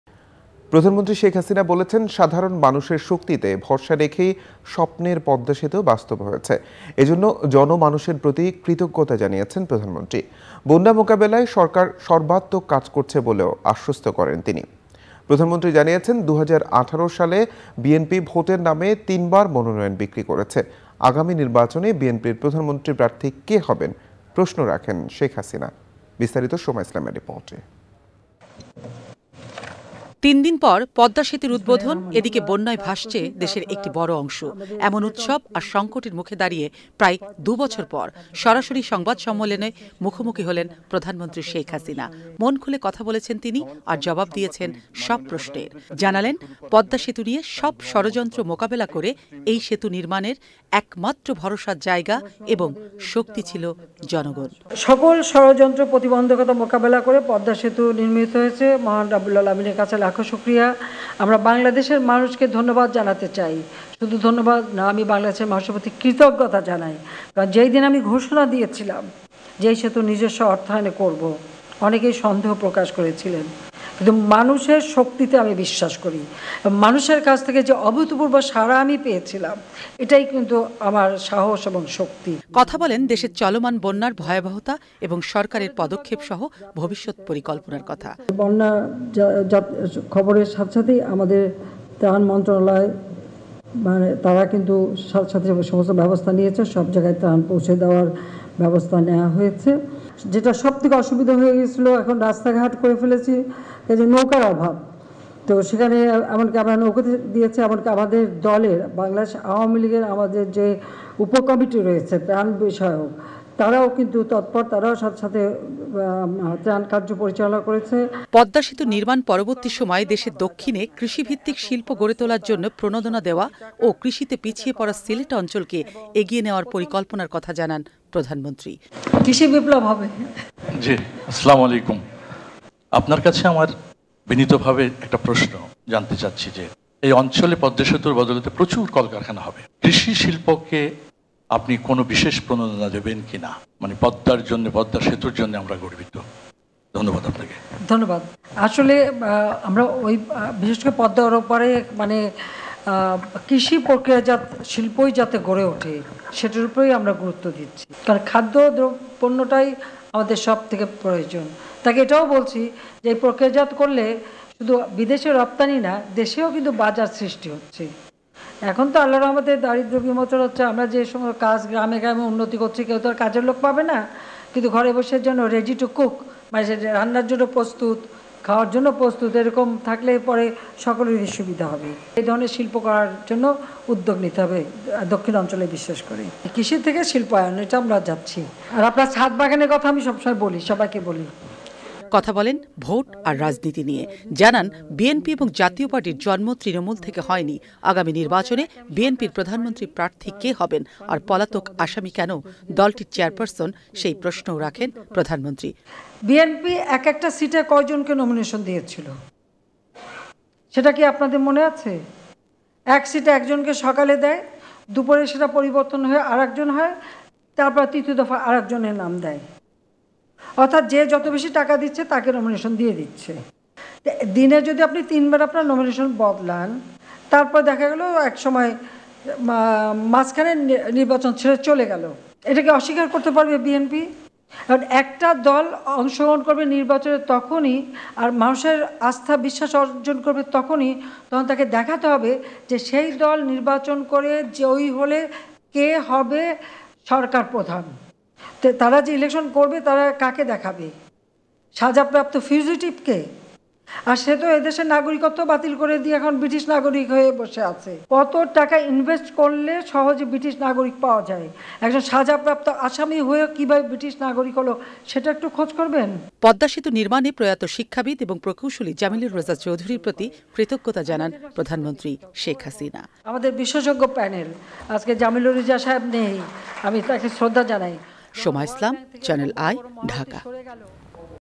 আগামী নির্বাচনে বিএনপির প্রধানমন্ত্রী প্রার্থী কে হবেন: প্রধানমন্ত্রী
প্রধানমন্ত্রী শেখ হাসিনা বলেছেন, সাধারণ মানুষের শক্তিতে ভরসা রেখেই স্বপ্নের পদ্মা সেতু বাস্তব হয়েছে। এজন্য জনমানুষের প্রতি কৃতজ্ঞতা জানিয়েছেন প্রধানমন্ত্রী। বন্যা মোকাবেলায় সরকার সর্বাত্মক কাজ করছে বলেও আশ্বস্ত করেন তিনি। প্রধানমন্ত্রী জানিয়েছেন, ২০১৮ সালে বিএনপি ভোটের নামে তিনবার মনোনয়ন বিক্রি করেছে। আগামী নির্বাচনে বিএনপি’র প্রধানমন্ত্রী প্রার্থী কে হবেন? প্রশ্ন রাখেন শেখ হাসিনা। বিস্তারিত সোমা ইসলামের রিপোর্টে। (আপডেট)

worldwide.